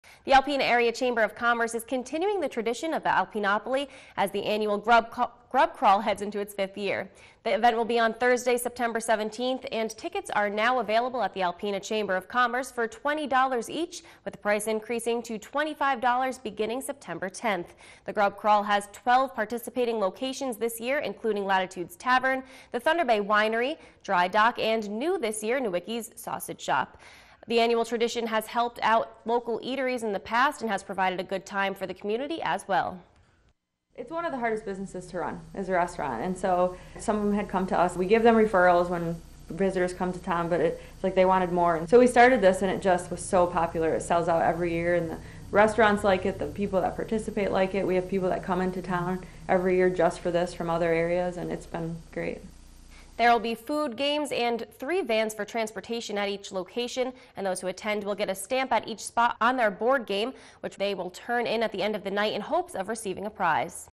Time to Pick Up Alpenopoly Tickets
The Alpena Area chamber of commerce is continuing the tradition of Alpenopoly, as the annual grub crawl heads into its fifth year.